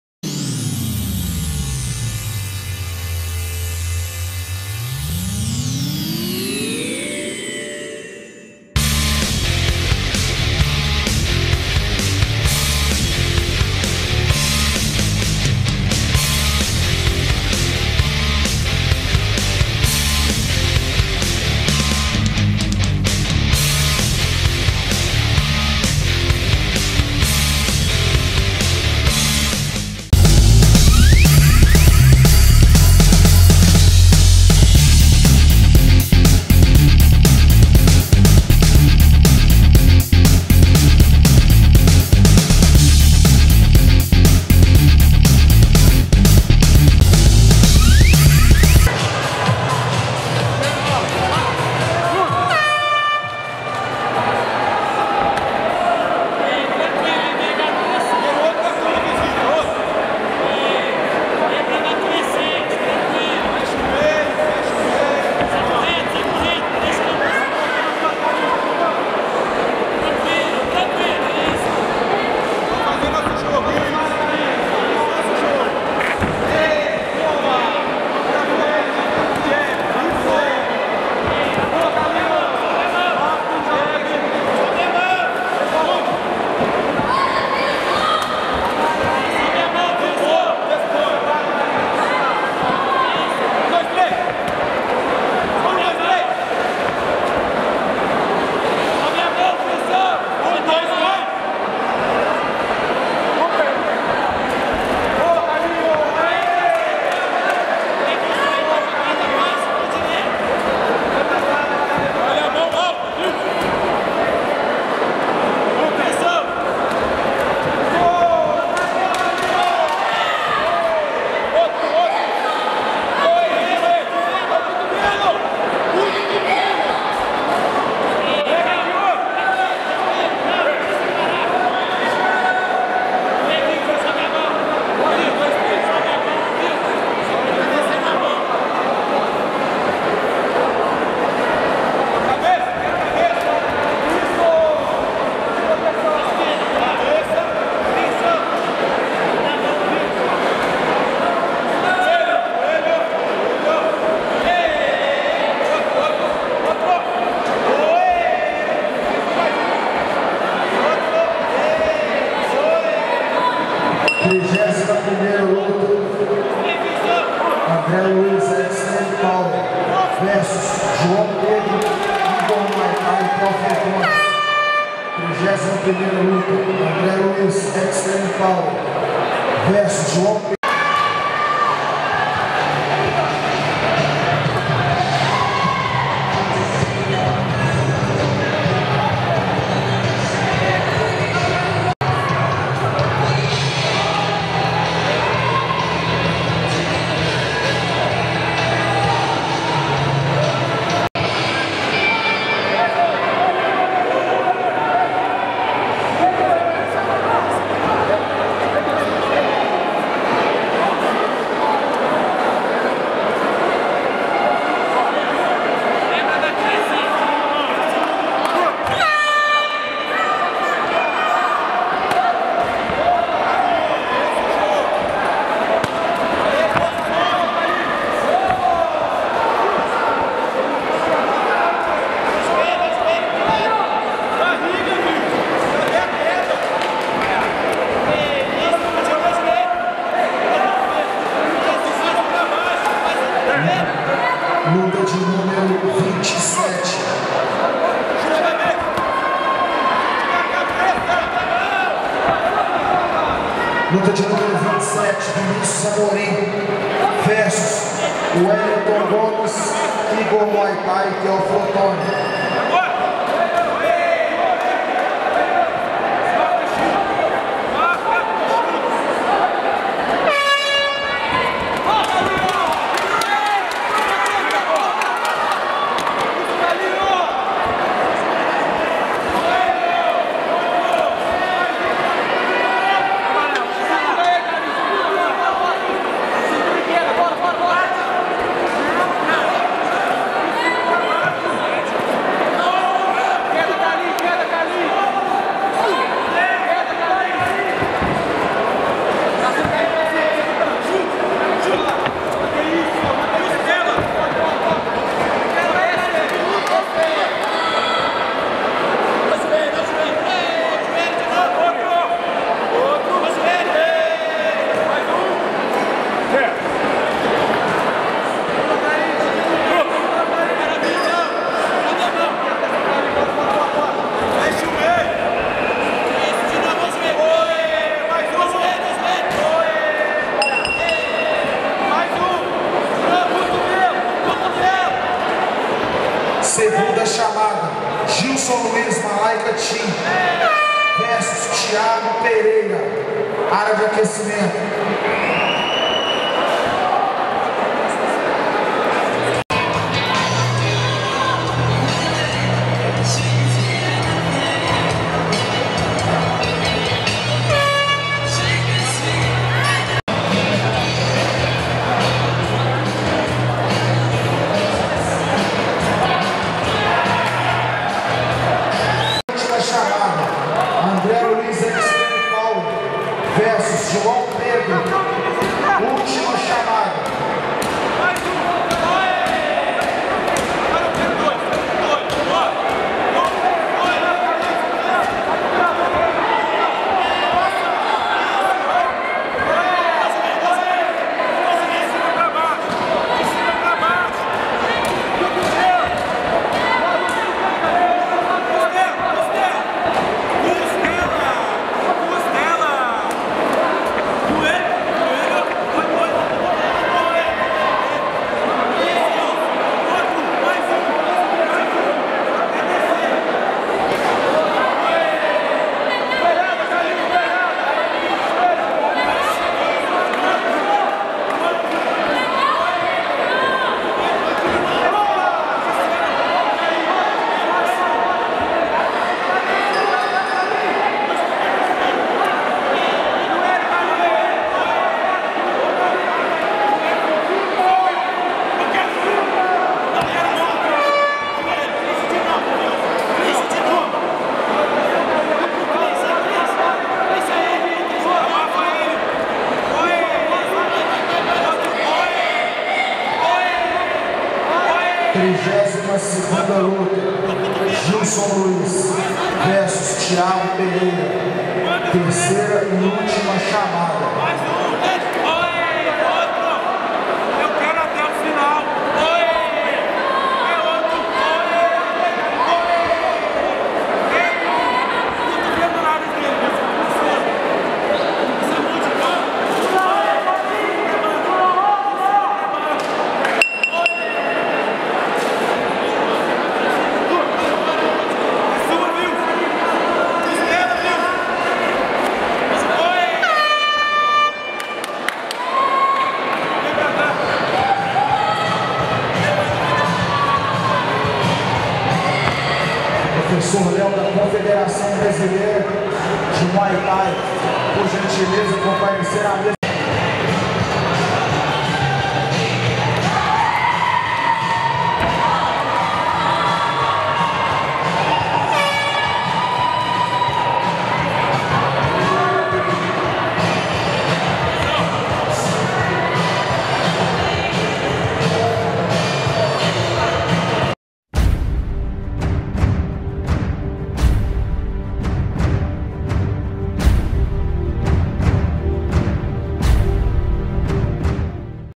Vilson vs Antonio - Campeonato Mineiro de Muay Thai 2018
Vilson Rodrigues venceu por pontos Antonio Carlos - Campeonato Mineiro de Muay Thai 2018